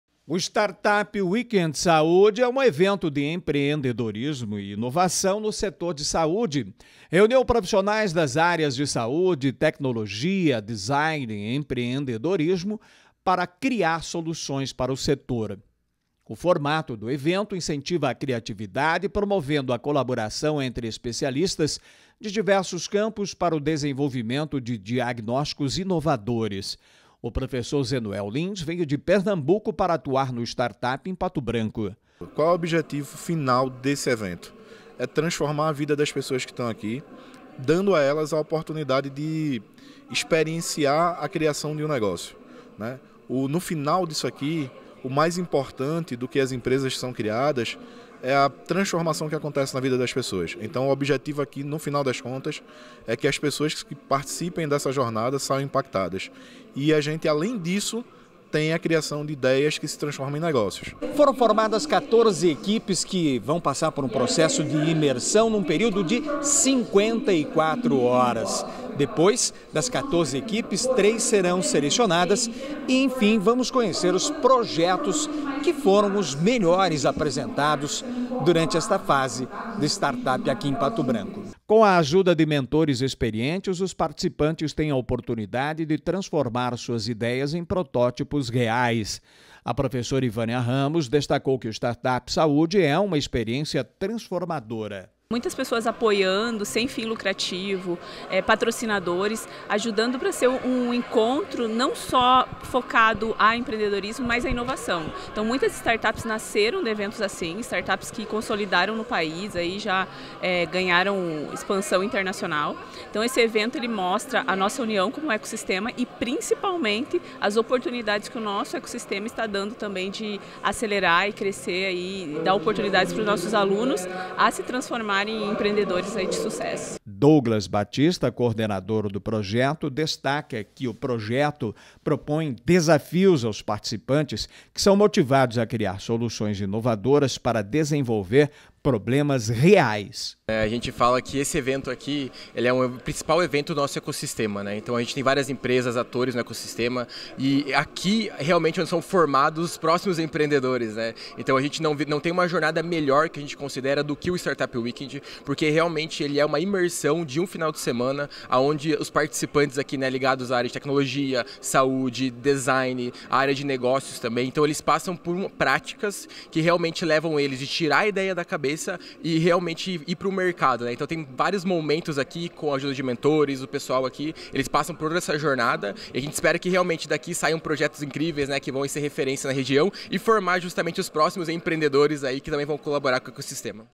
Empreendedores realizam imersão de 48 horas em Startup sobre saúde
Eventos: A Sudovaley, comunidade de Startups do Sudoeste do Paraná, promoveu uma imersão empreendedora na Startup Weekend Saúde, durante o final de semana.